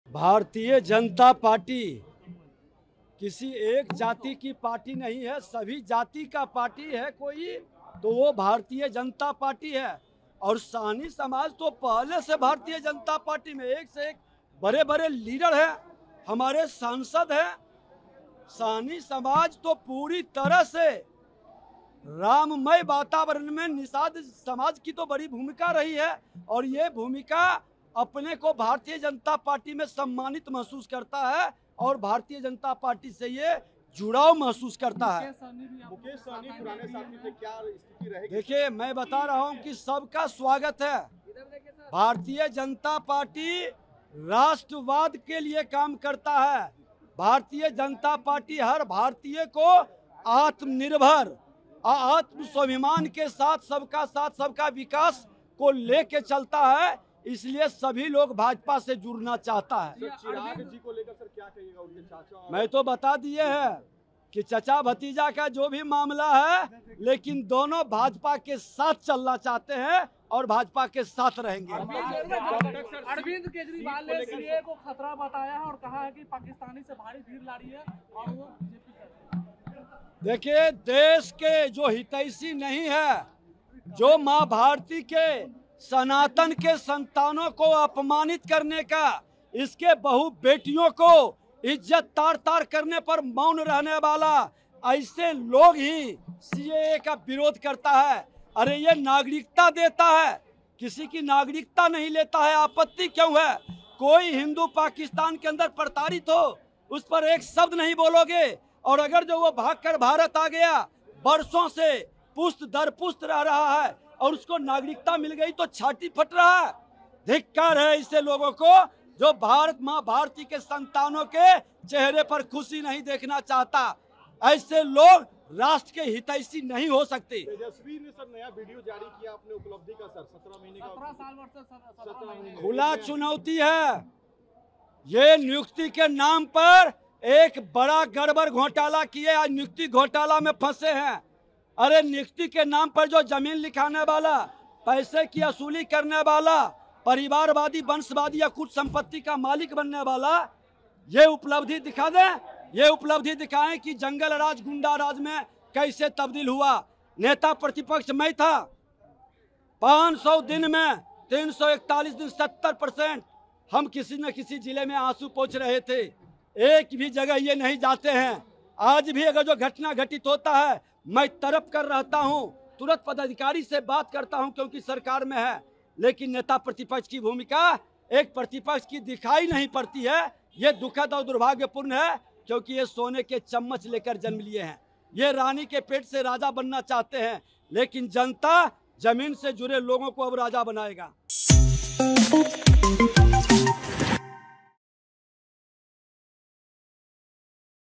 Mukesh Sahni के BJP के साथ आने के सवाल पर Vijay Sinha ने कुछ इस अंदाज में दिया जवाब
बिहार के डिप्टी सीएम विजय सिन्हा ने बताया कि बीजेपी पार्टी किसी एक वर्ग की पार्टी नहीं है ये सभी वर्गों की पार्टी है. बीजेपी पार्टी से सहनी समाज जुड़ा हुआ है... मुकेश साहनी पर उन्होंने कहा कि बीजेपी पार्टी राष्ट्रवाद पर काम करती है इसलिए ही सभी हमसे जुड़ना चाहते है। जो हमारे साथ आना चाहता है उनका स्वागत है...